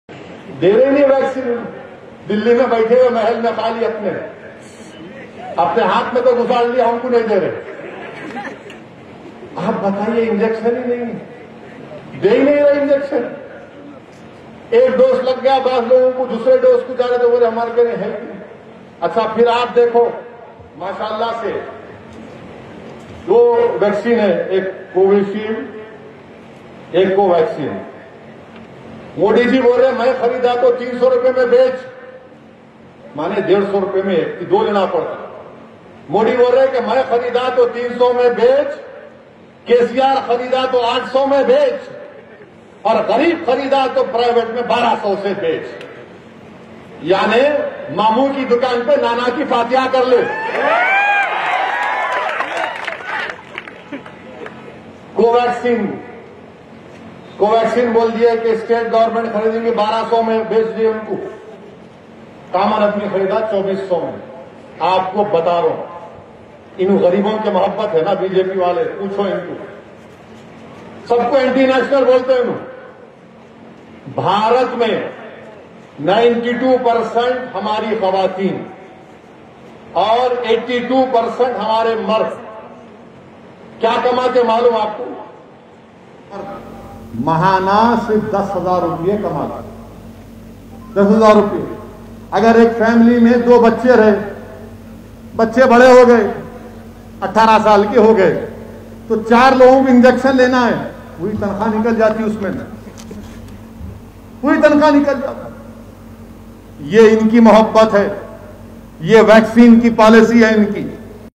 Vaccine policy ka toh aisa masla hai jaise Mamu ki Dukan per Nana ki Fateha. Central Government -
Vaccine policy ka toh aisa masla hai jaise Mamu ki Dukan per Nana ki Fateha.
Central Government - 300₹ per 2 dose
State Government - 800₹ per 2 dose
Private Medical - 1200₹ per 2 dose - Barrister Asaduddin Owaisi

All India majlis-e-ittehadul muslimeen

My name ARBAZ SHAIKH
house India Maharashtra jila Ahmednagar Shrirampur Shahar taluka  🏘️